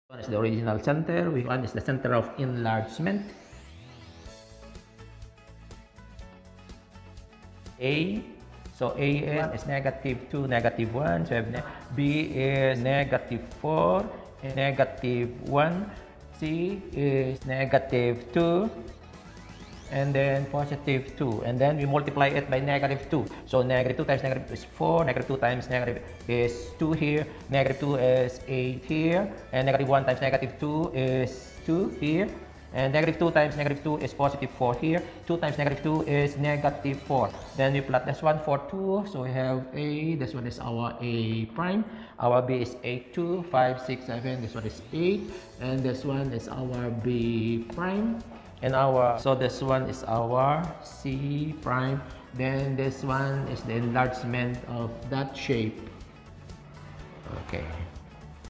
Transformation by Enlargement by a negative factor.
This video explains how to draw transformation by enlargement by a negative factor.